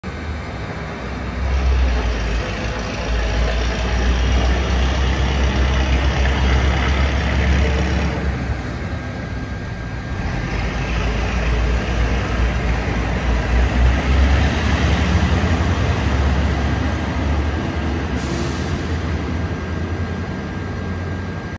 Lättähatut lähtevät yöhön Lahdesta
Porvoon Museorautatie ry:n Dm 7 -museojuna lähtee lokakuisen perjantai-illan jo hämärryttyä Lahden asemalta kohti Kouvolaa.